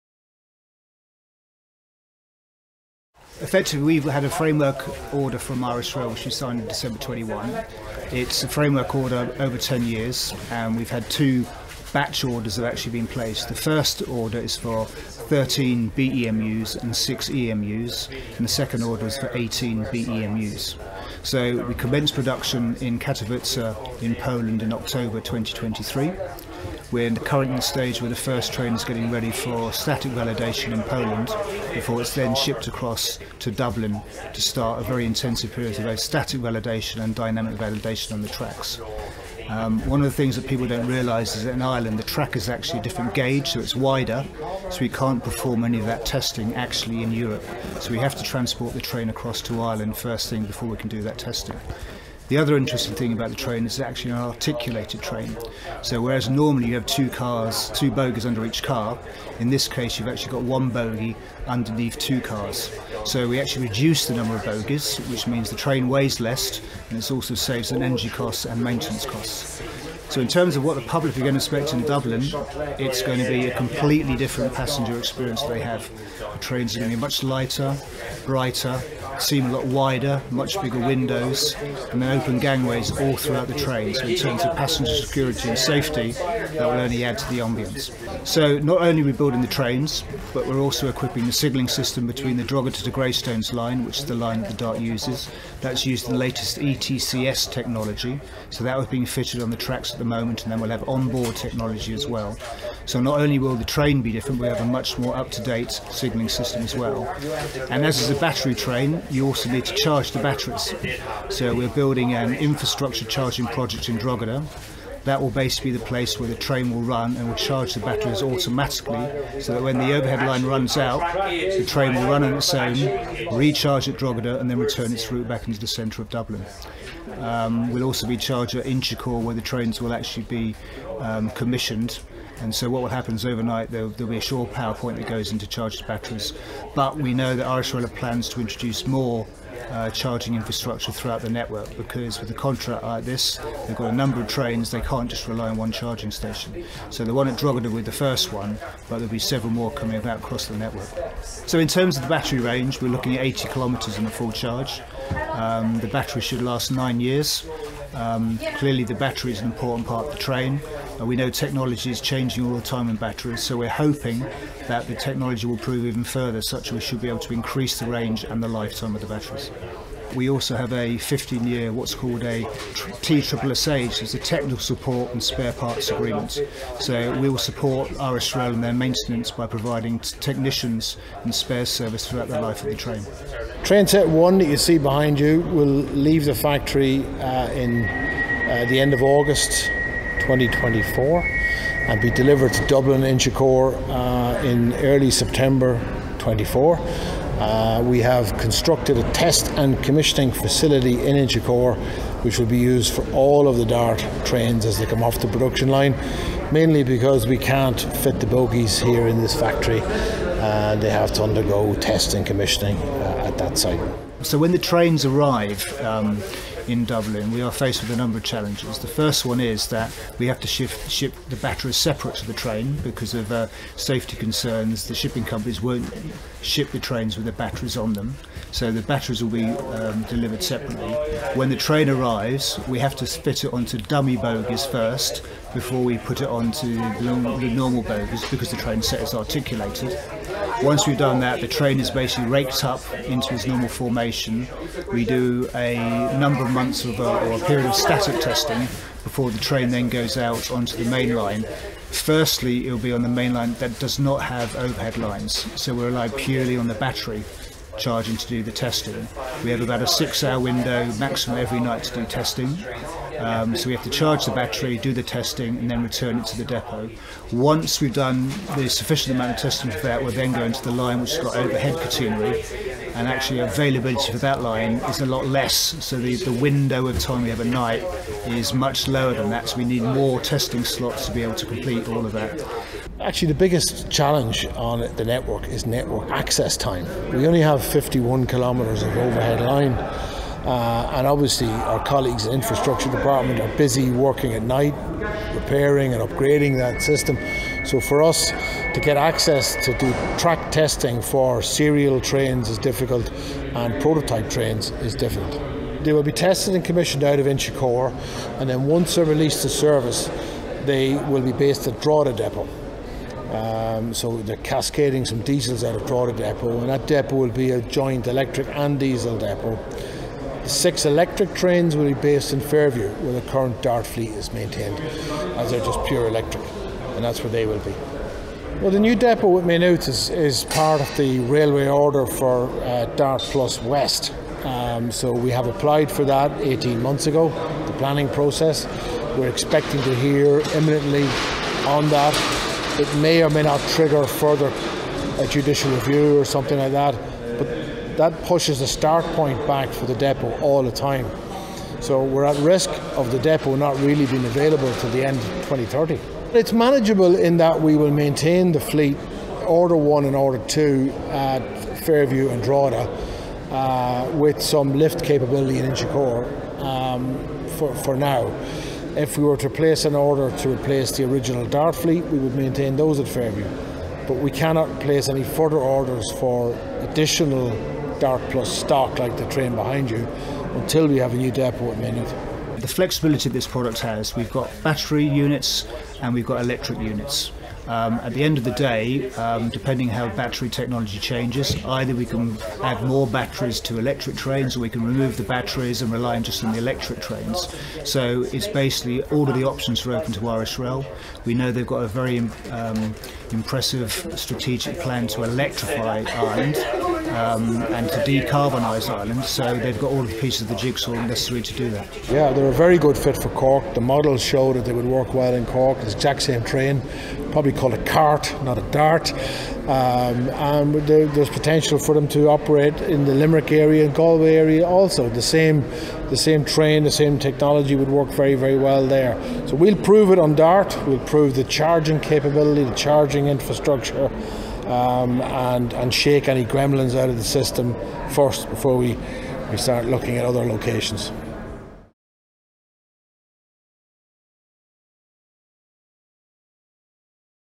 First Dublin DART+ battery-electric multiple-unit
Metro Report International visits Altom's factory at Katowice in Poland to see the first battery-electric multiple-unit for Dublin's DART+ commuter railway electrification and enhancement programme.